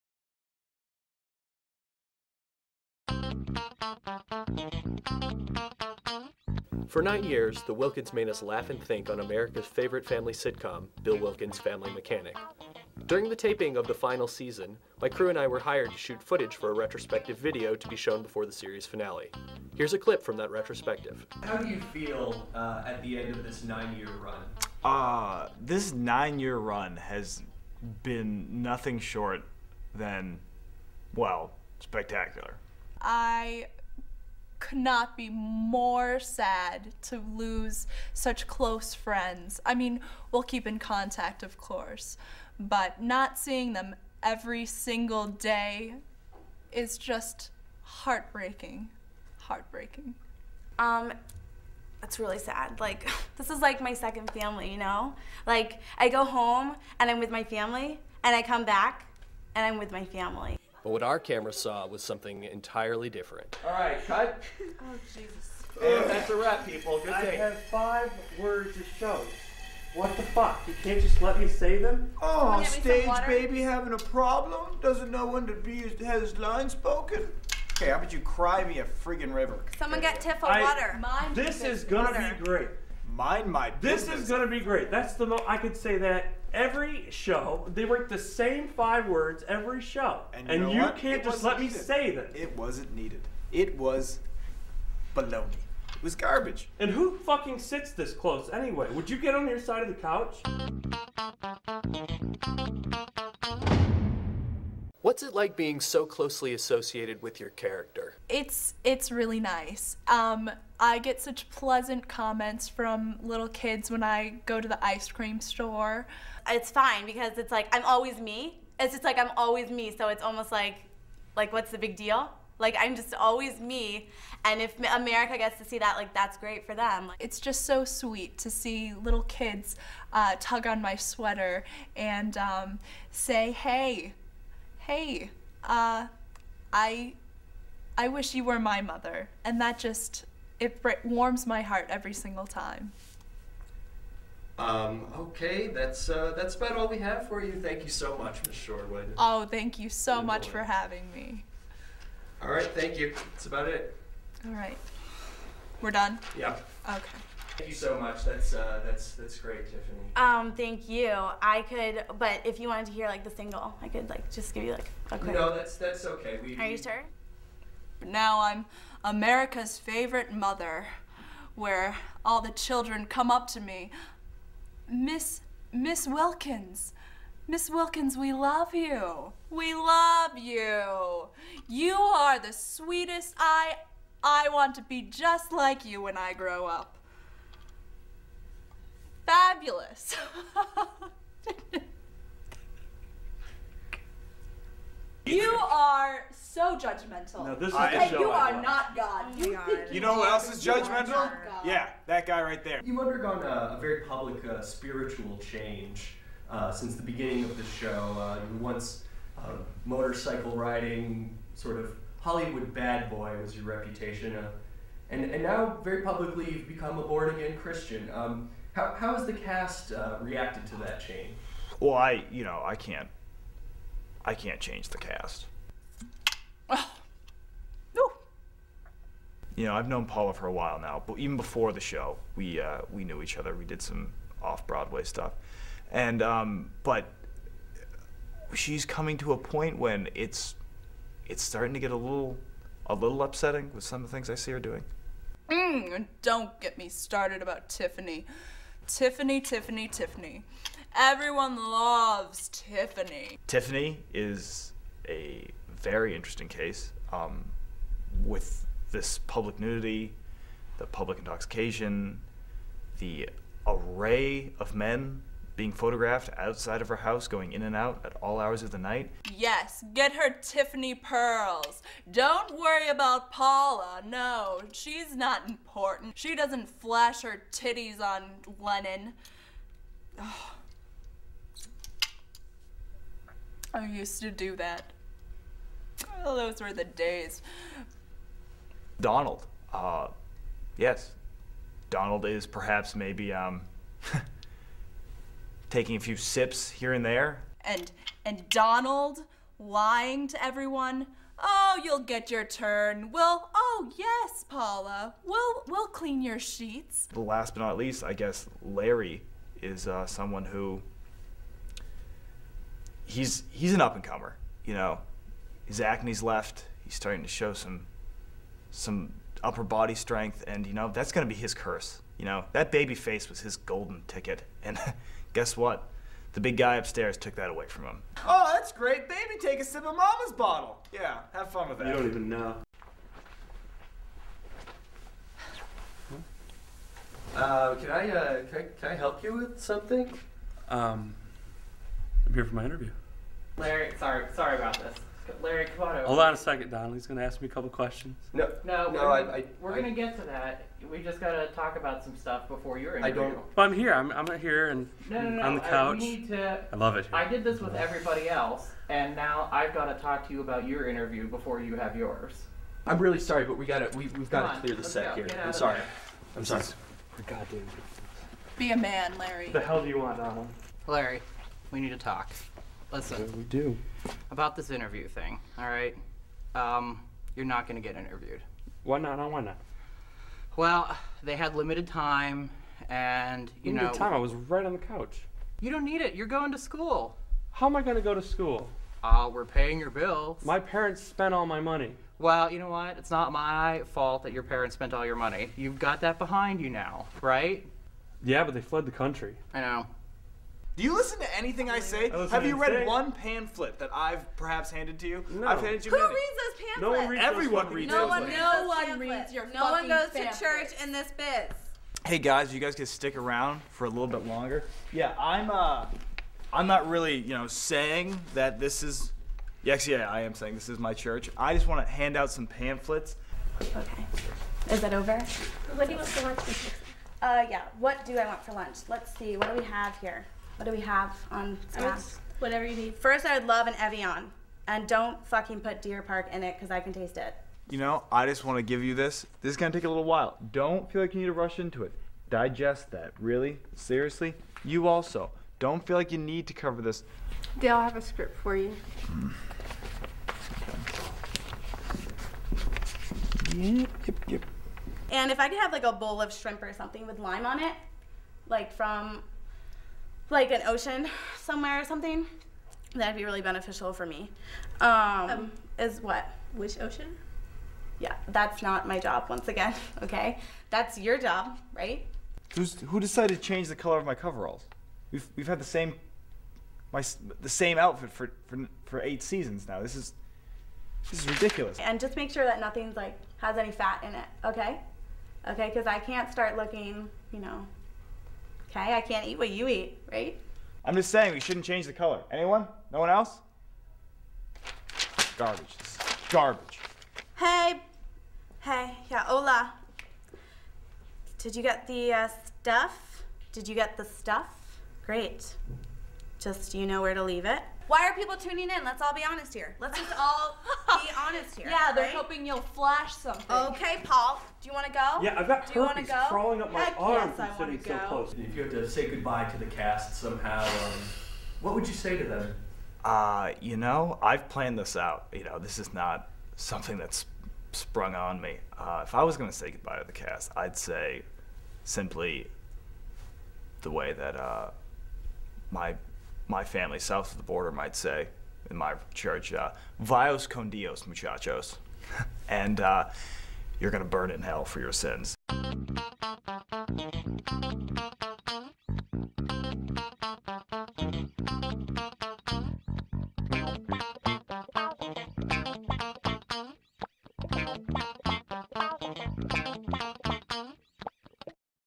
Bill Wilkins Confidential Episode 1: Pilot
An introduction to the cast of America's favorite family sitcom, "Bill Wilkins: Family Mechanic"